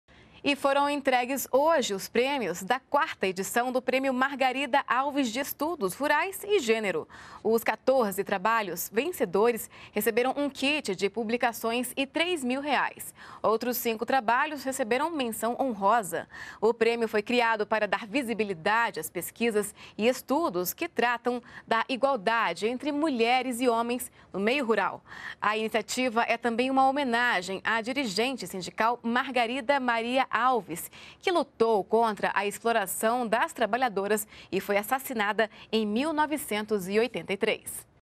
Prêmio valoriza pesquisas referentes a igualdade de gênero no meio rural
NBR NOTÍCIAS - 09.12.14: Nesta terça-feira (9), foram premiados os vencedores da 4ª edição do Prêmio Margarida Alves de Estudos Rurais e Gênero, promovido pelo Ministério do Desenvolvimento Agrário (MDA). Os 14 trabalhos selecionados receberam um kit de publicações e o valor de R$ 3 mil. Outros cinco trabalhos receberam menção honrosa. O prêmio foi criado para dar visibilidade as pesquisas e estudos referentes a igualdade entre mulheres e homens no meio rural. A iniciativa é uma homenagem a dirigente sindical Margarida Maria Alves, que lutou contra a exploração das trabalhadoras e foi assassinada em 1983.